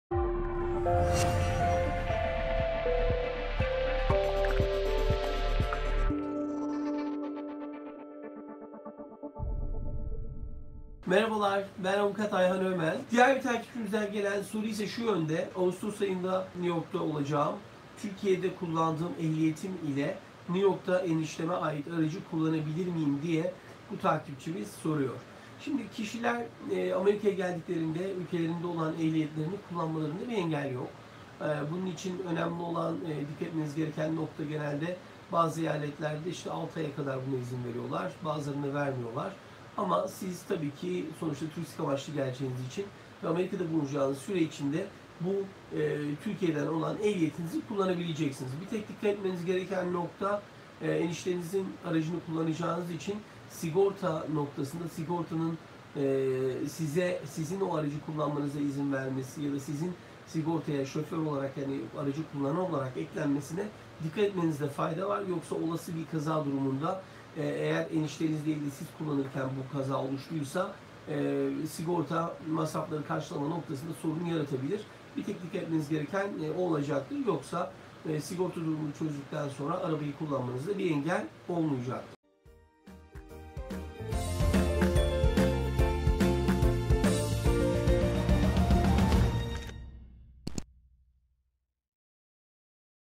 TÜRKİYE EHLİYETİ İLE AMERİKA'DA ARAÇ KULLANILIR MI ?
Soru: Ağustos ayında New York'ta olacağım.  Türkiye'de kullandığım ehliyetim ile New York'ta enişteme ait aracı kullanabilir miyim ?